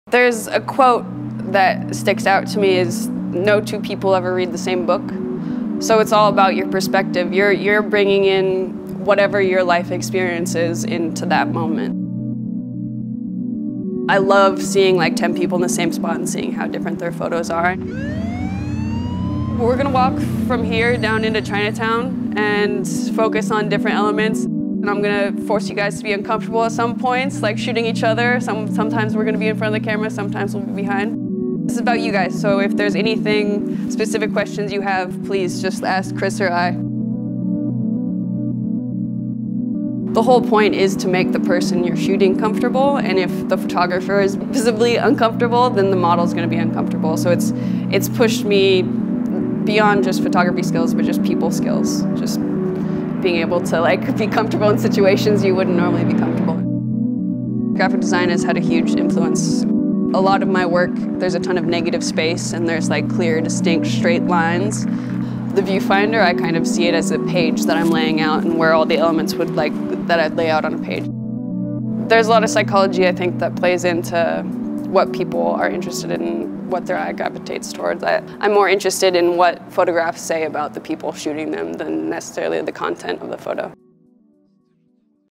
Laura Austin Leads a Vision Walk in Los Angeles
We are traveling around the country with Vans Vision Walks, a series of workshops led by some of our favorite photographers in their home cities. This past weekend we spent the afternoon with Laura Austin as she led participants on a walk through Chinatown.

Video: Justin Carlson
Photos: Tatum Anderson, Leilani Molina, Laura Austin